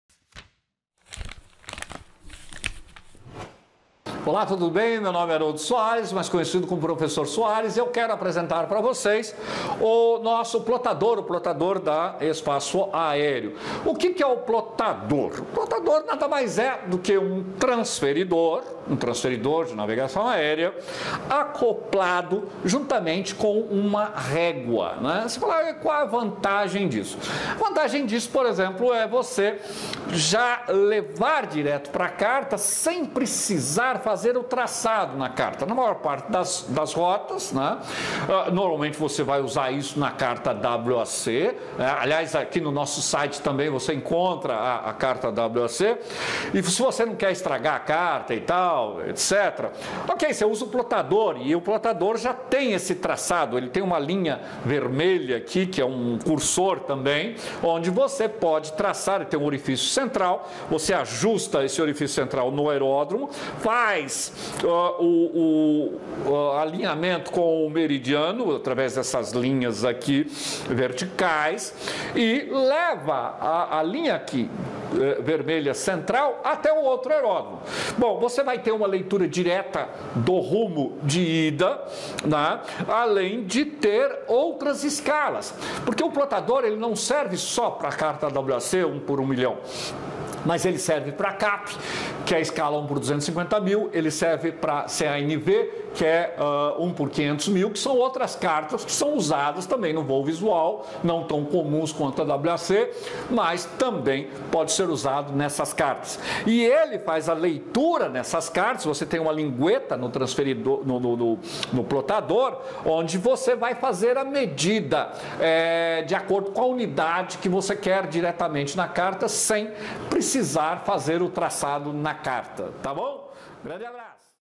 Plotador - Espaço Aéreo
Vantagens do Plotador:
- Não há a necessidade de riscar a carta.
- Além de possuir a escala para cartas WAC (1:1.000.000, o plotador também tem escala para as cartas CNAV (1:500.000) e CAP (1:250.000).
- Calcula rumo de ida e volta.
Indicado para todo aluno que for realizar o curso de piloto privado (PP), ou piloto comercial e instrumentos (PC/ IFR) em Universidades, Centros de treinamento, Escolas de aviação e Aeroclubes, ou ainda, para pilotos em geral.